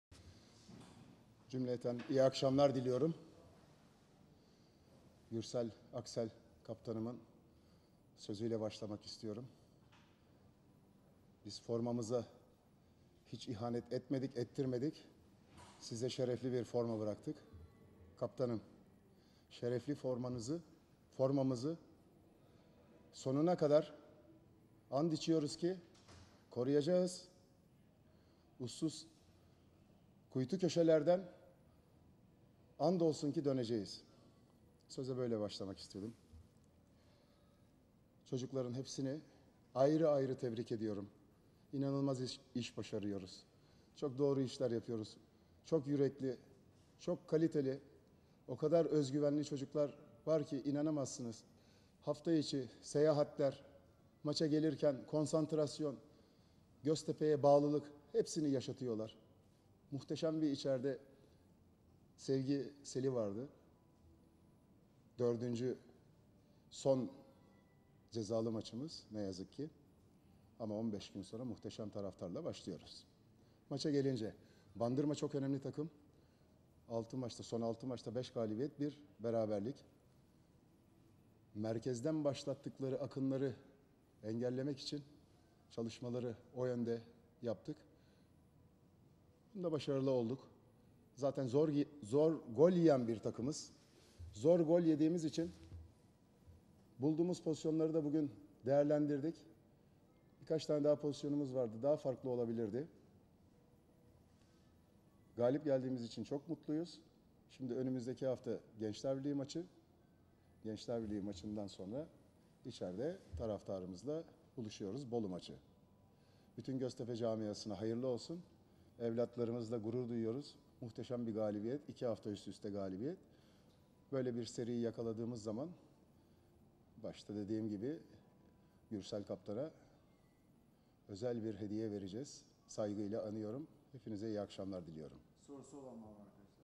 Şakir Özkayımoğlu: “Doğru işler yapıyoruz”
Göztepe Teknik Direktörü Şakir Özkayımoğlu, Bandırmaspor maçının ardından çok doğru işler yaptıklarını ifade ederek, “Oyuncular çok konsantre, soyunma odasında inanılmaz bir sevinç vardı” şeklinde konuştu.